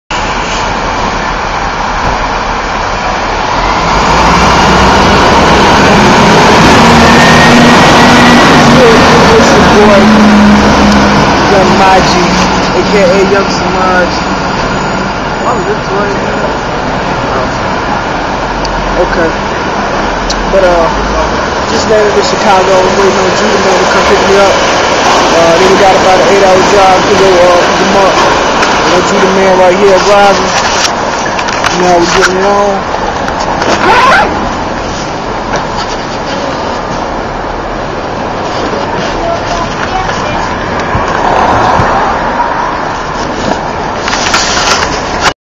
the life of yung semaj pt.28
yung semaj waiting to be picked up in CHICAGO going 2 memphis,tn to do a concert w/MR.DEL